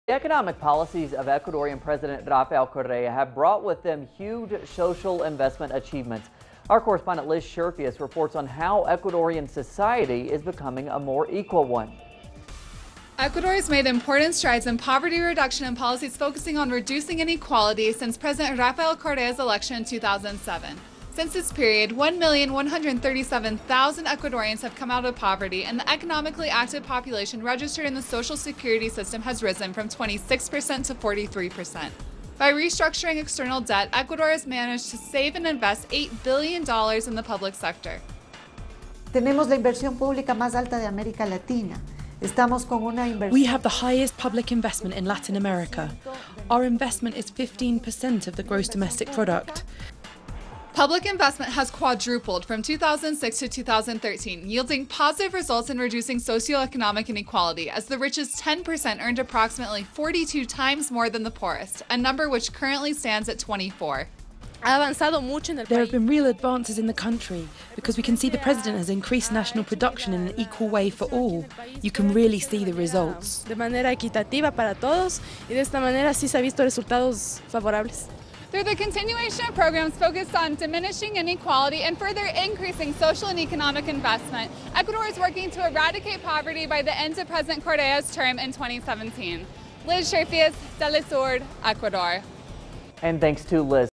Ecuador: social investment translates into achievements
Ecuadorian economic policies under President Rafael Correa have resulted in huge advances for the population and there is less inequality thanks to social investment. Such social investment now represents 15% of GDP, a four fold increase since 2006. In addition, over 1.100 million Ecuadorians no longer live in poverty, and the percentage of the economically active population enrolled in the social security and health care system has increased from 26% to 43%. Liz Sherffius reports from Quito. teleSUR